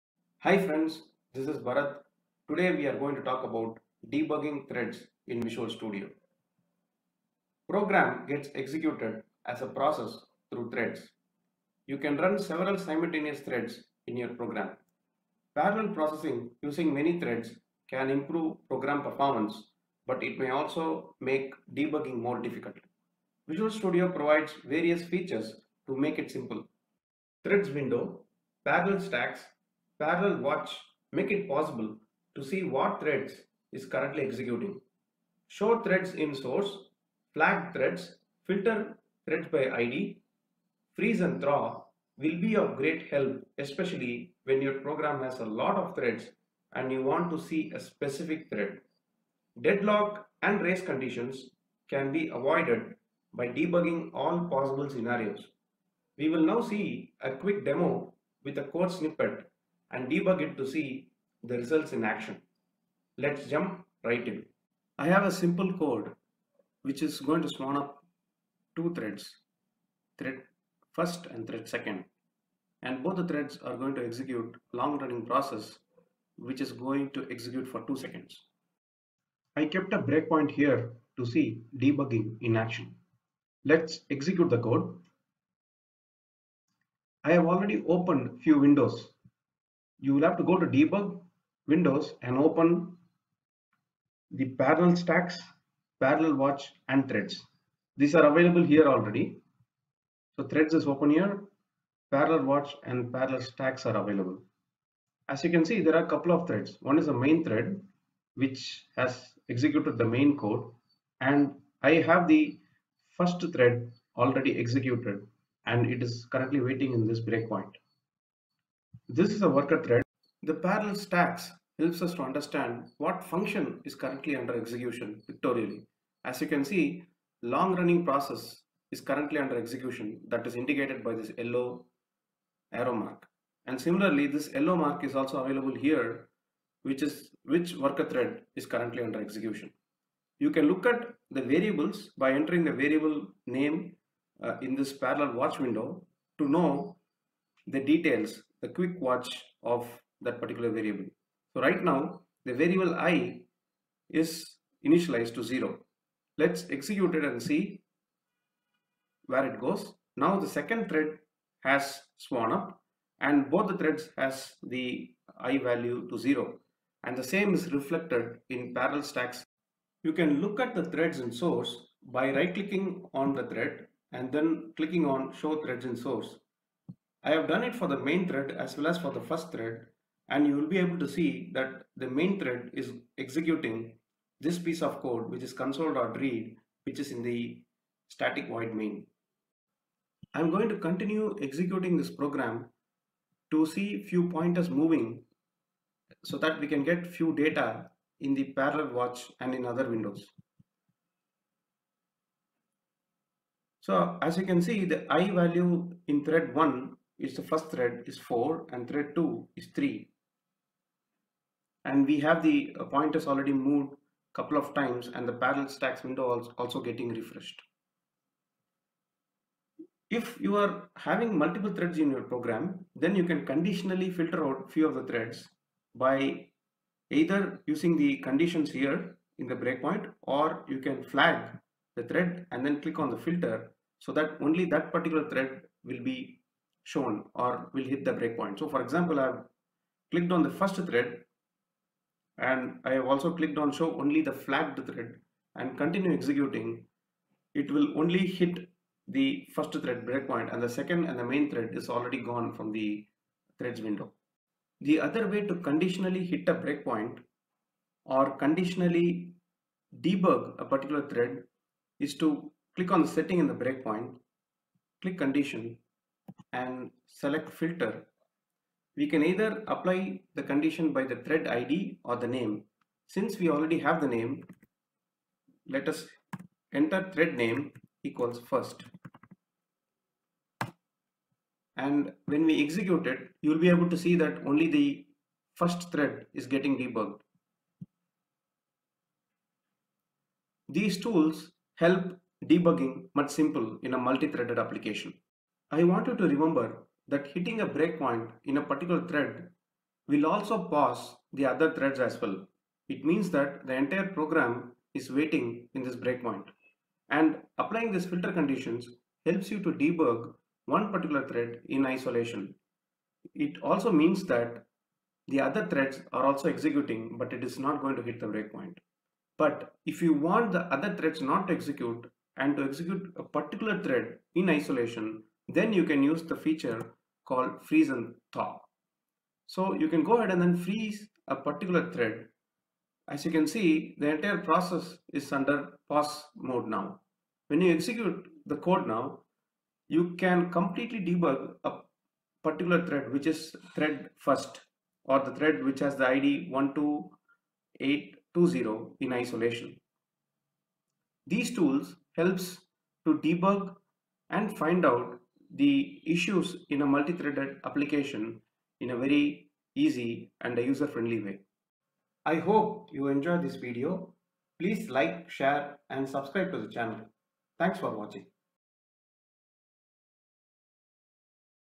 Debug Threads in Visual Studio 2019
Program gets executed as a process through threads. You can run several simultaneous threads in your program. Parallel processing using many threads can improve program performance, but it may also make debugging more difficult. Visual studio provides various features to make it simple. Threads Window, Parallel Stacks, Parallel Watch make it possible to see what thread is currently executing. Show Threads in source, Flag Threads, Filter by thread id, Freeze & Thaw will be of great help especially when your program has a lots of threads and you want to see a specific thread. Dead local and race conditions can be avoided by debugging all possible scenarios. We will now see a quick demo with a code snippet, and debug it to see the results in action. Let's jump right in.